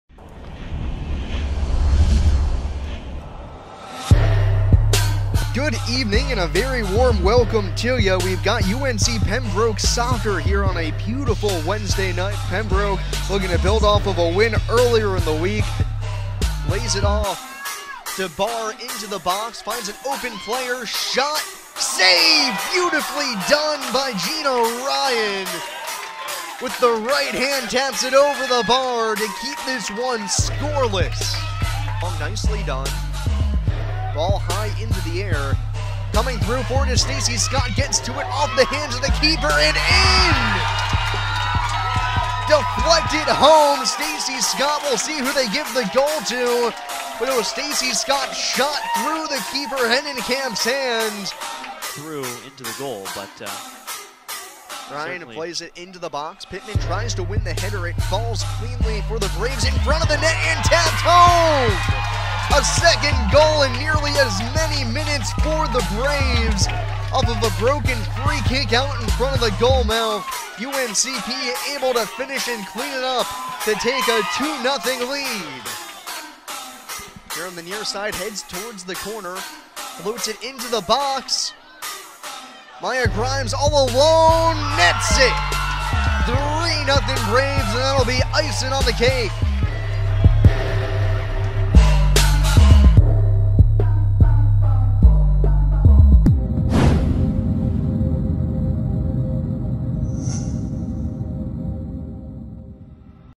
Braves Soccer Highlights: UNCP vs Francis Marion, 10/17/18
Watch full highlights from the Braves' 3-0 victory over Frances Marion (5-7-1, 2-5 PBC). UNCP (7-4, 4-3 PBC) won its third consecutive match and stayed unbeaten at home in 2018.